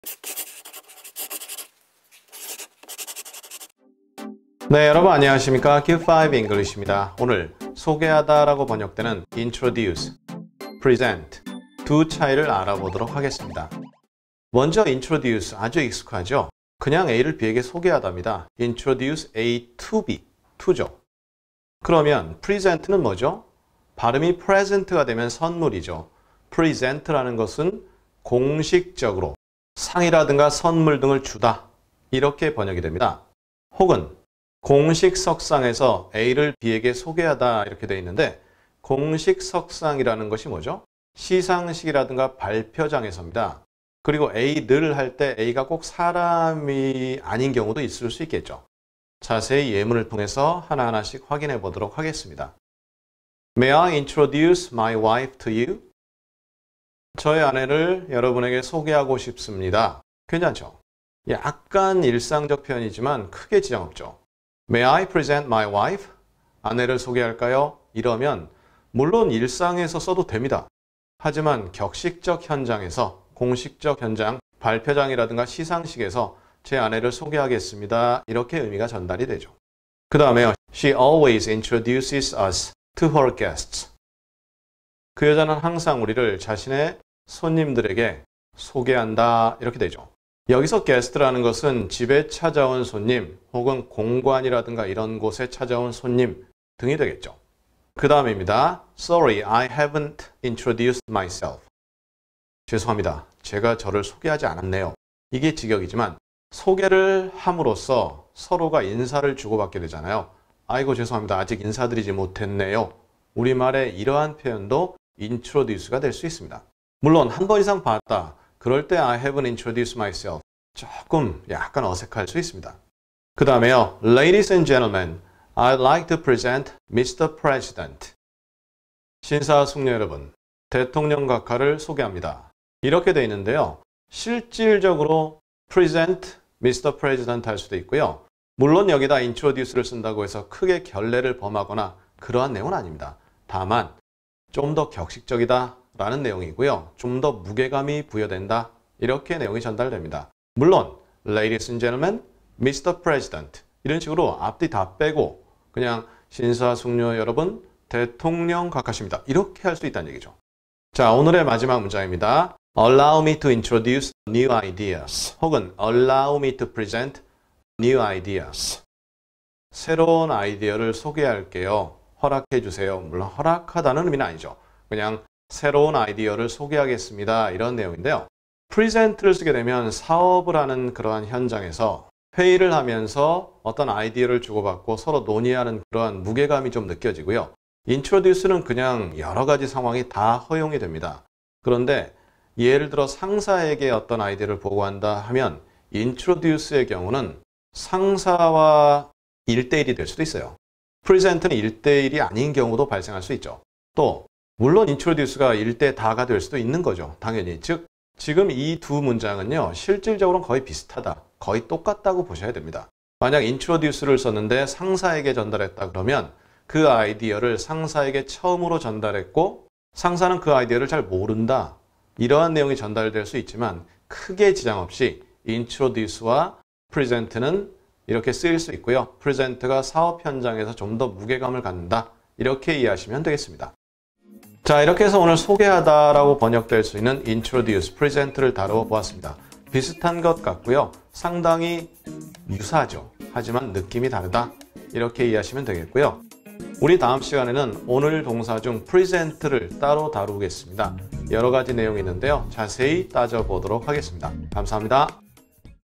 Q5 English 어휘 Lesson 1366 (소개하다 – introduce vs present)
오늘은 소개하다 라는 일반적 의미입니다.  의미도 큰 차이 없습니다.  다만 격식적인 면에서 present가 더 높다는 것입니다.  하지만 일상에서 써도 크게 지장 없습니다.  오늘 내용 자세히 확인해 보시죠.  감사합니다.